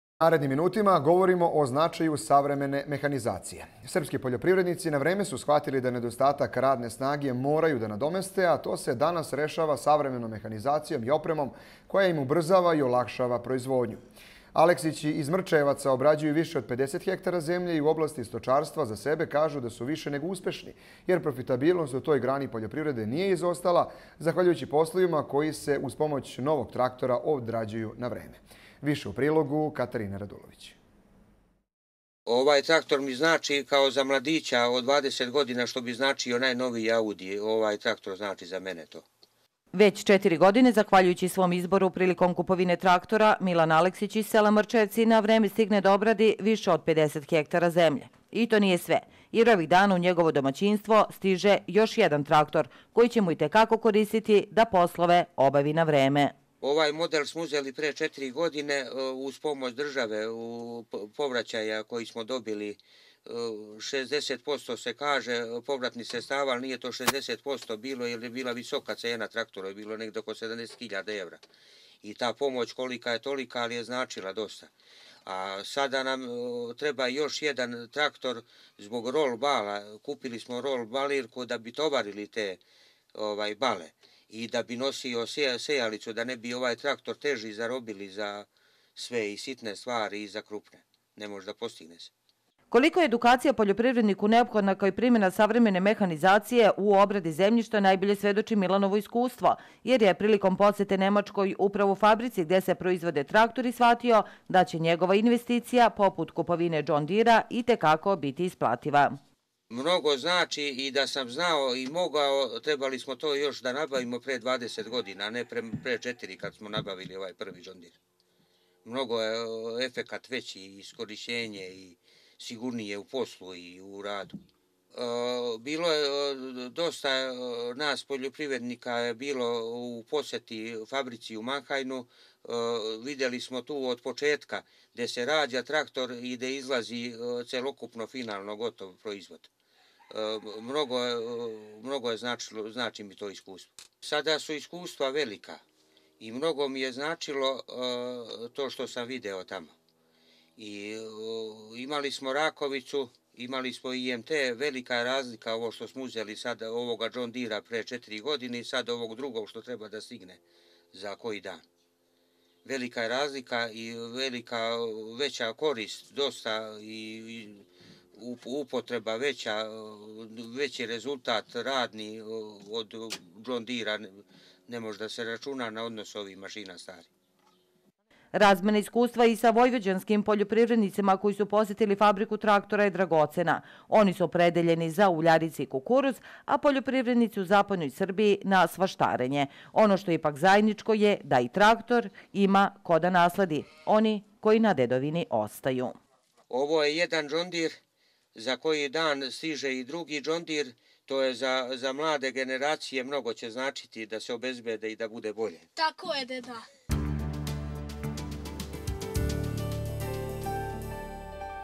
Dobar traktor - spas za domaćina
Priča o značaju savremene mehanizacije u poljoprivredi iz ugla Milana Aleksića, poljoprivrednika iz Mrčajevaca.